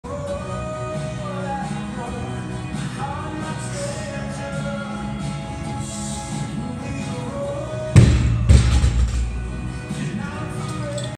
245 Thruster
So much fatigue today, holy balls. Felt a little off but it moved easy.